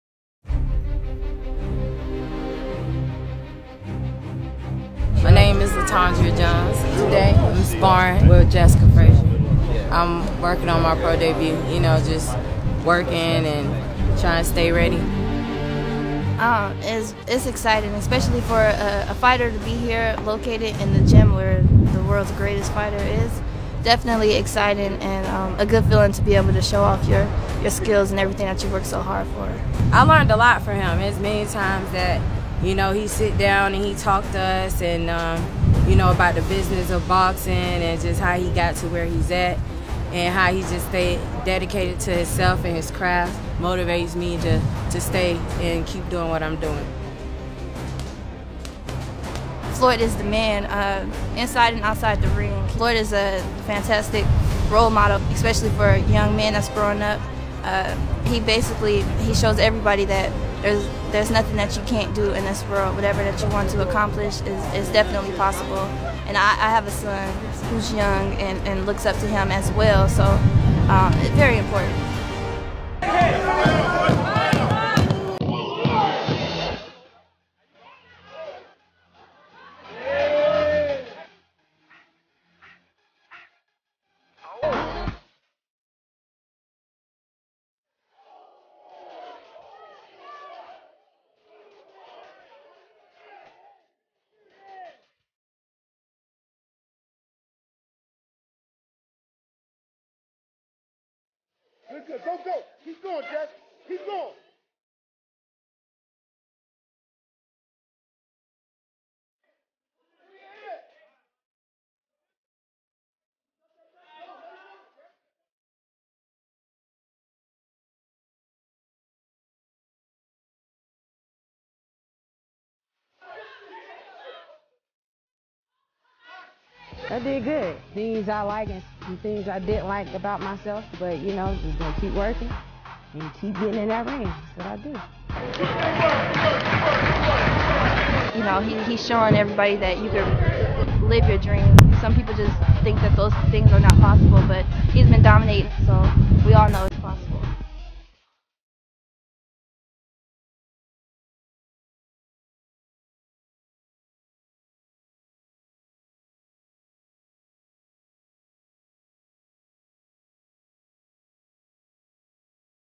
FLOYD MAYWEATHER INSPIRES FEMALE BOXERS AT MAYWEATHER GYM
A different side of Floyd Mayweather ahead is shown of his mega bout with Conor Mcgregor. Females boxers at Mayweather Promotions give props to Floyd for his guidance and leadership. It seems Floyd's history as an innovator will include an investment in female fighters.